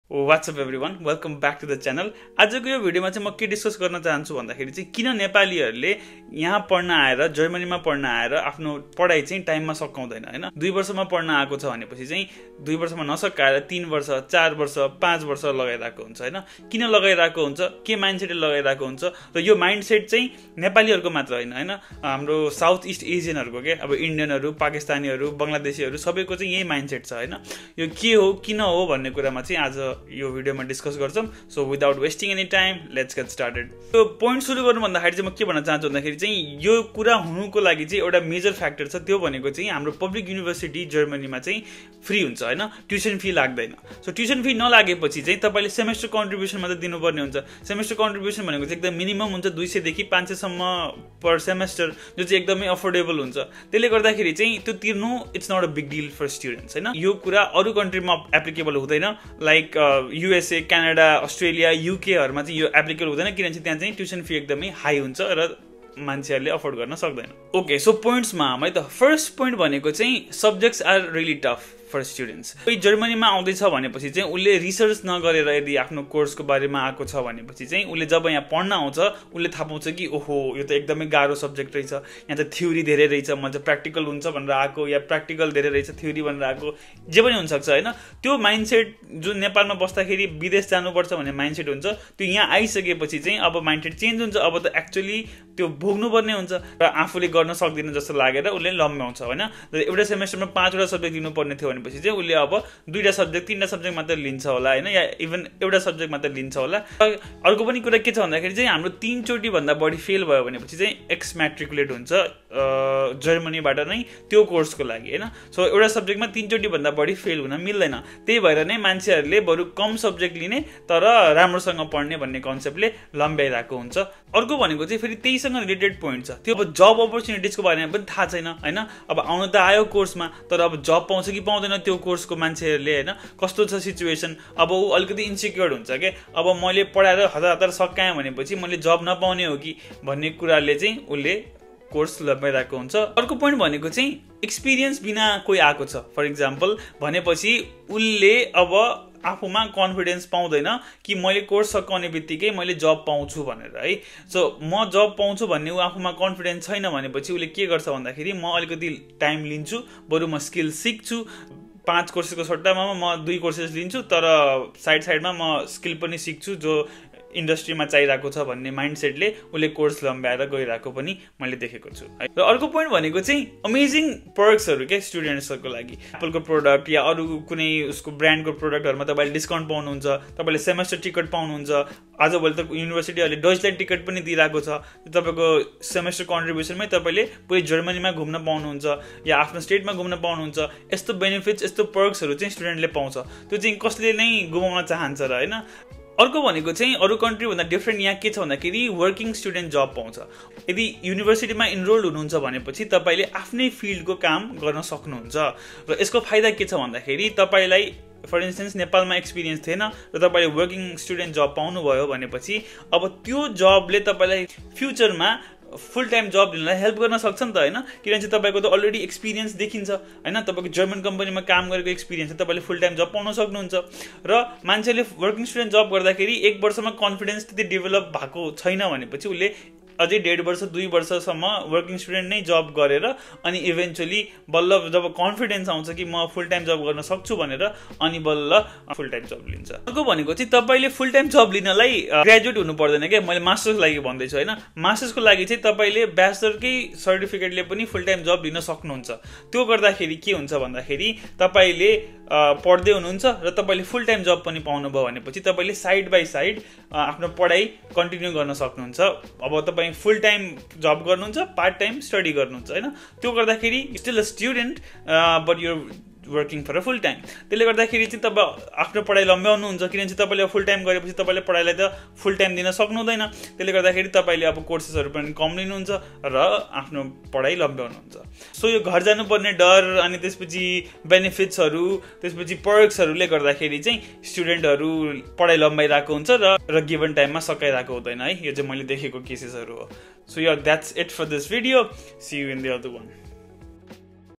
Why Nepalese students take longer to complete their studies?
In this video, we are going to talk about why Nepalese students take longer time than expected to complete their studies in Germany